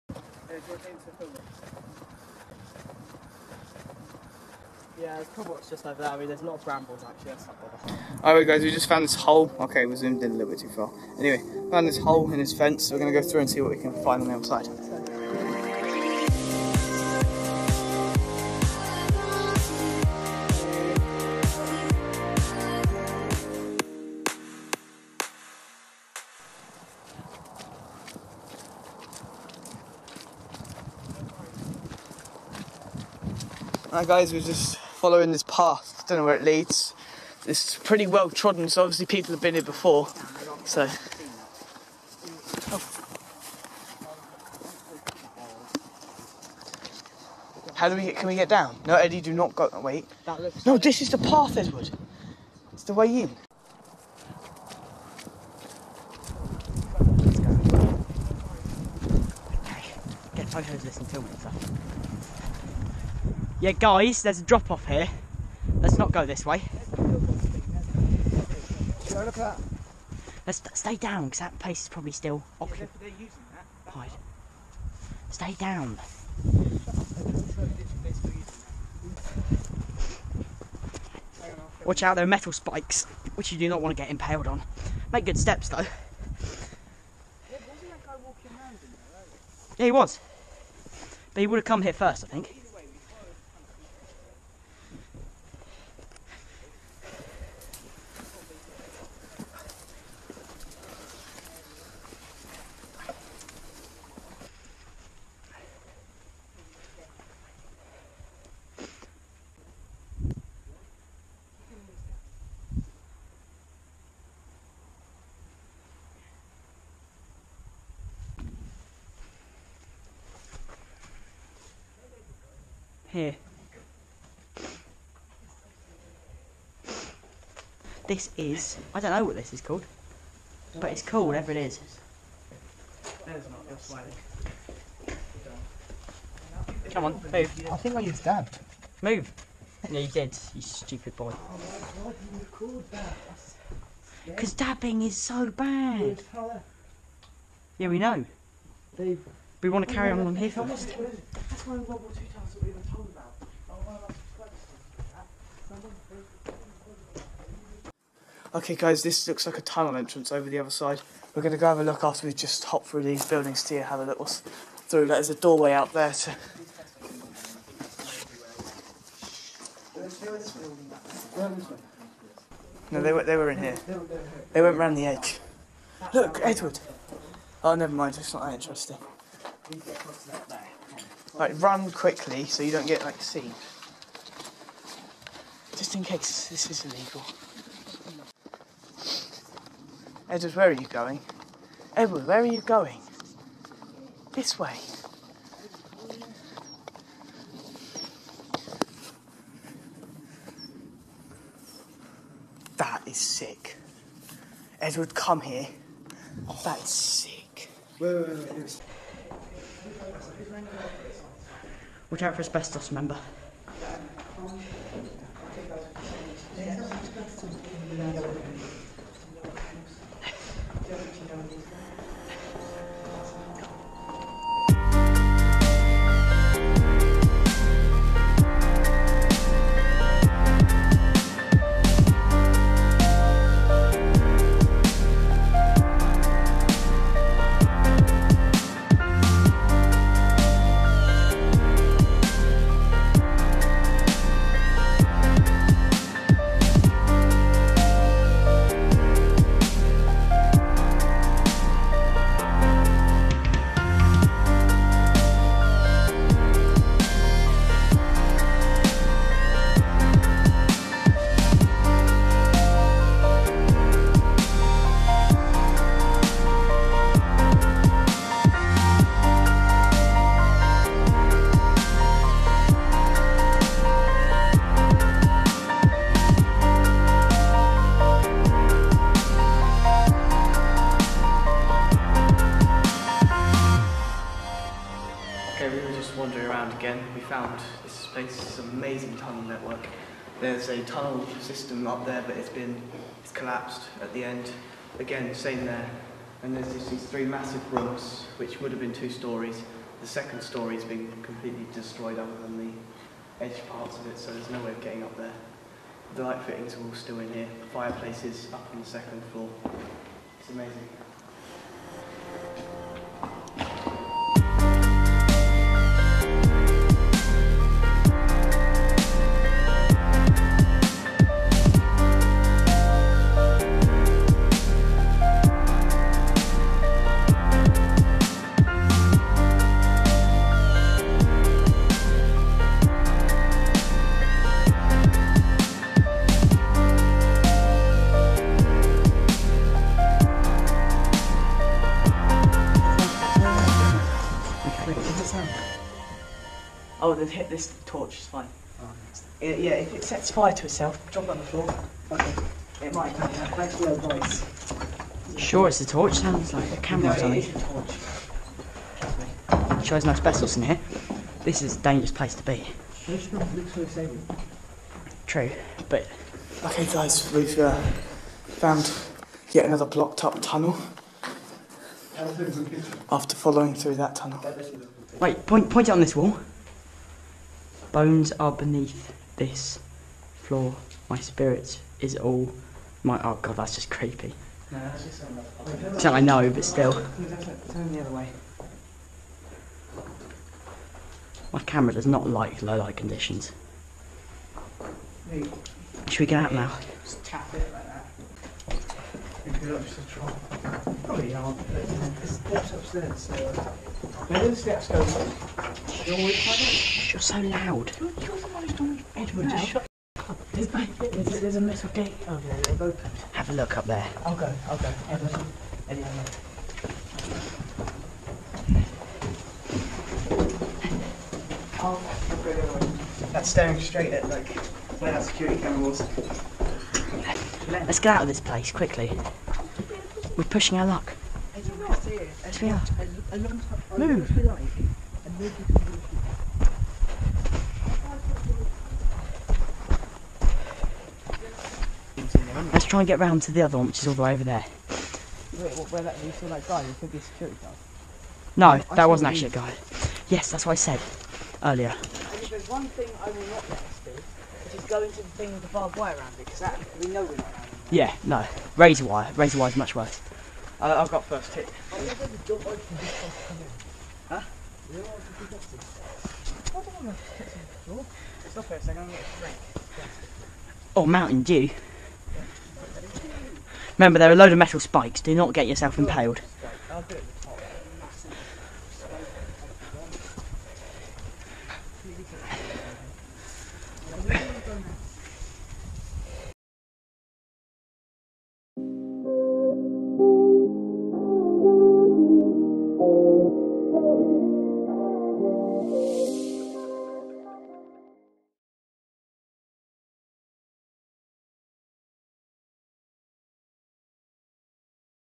THE SECRET TUNNELS OF CITADEL
THE SECRET TUNNELS OF KENT

Today we will be exploring a tunnel system in Kent, with huge rooms and a number of interesting finds...